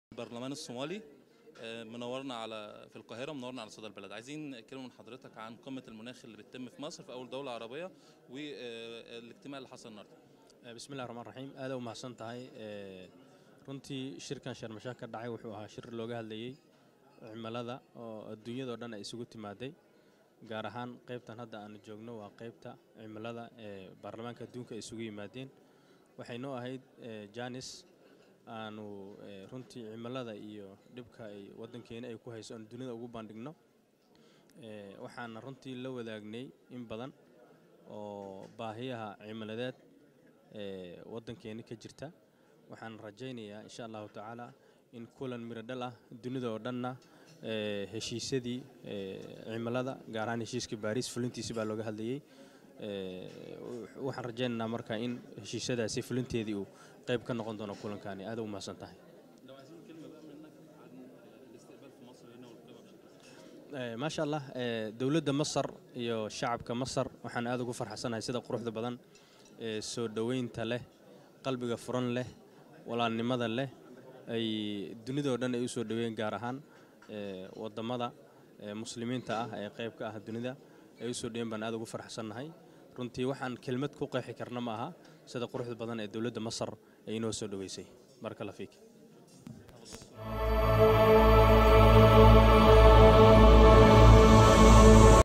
أصغر برلمانى صومالي يشكر مصر على حسن التنظيم بقمة المناخ
ElBaladOfficial#
ElBalad_Official#